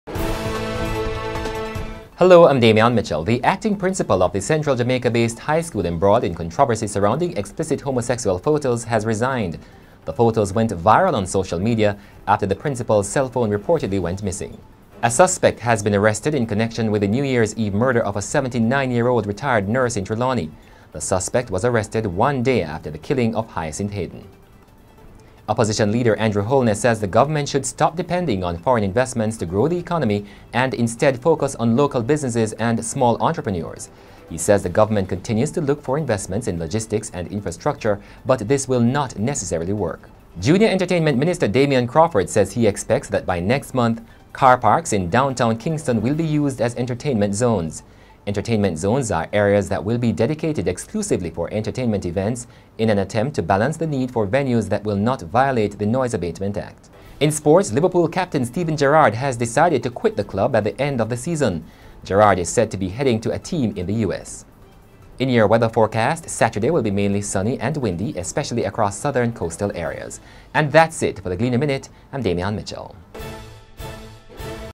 THE GLEANER MINUTE: Principal resigns … Car parks as entertainment zones … Steven Gerrard quits
JAN 2, 2015: The acting principal in controversy surrounding explicit homosexual photos has resigned … Andrew Holness says the Government should stop depending on foreign investments to grow the economy … Damion Crawford says he expects that by next month car parks in downtown Kingston will be used as entertainment zones … IN SPORTS:  Liverpool captain Steven Gerrard has decided to quit at the end of the season.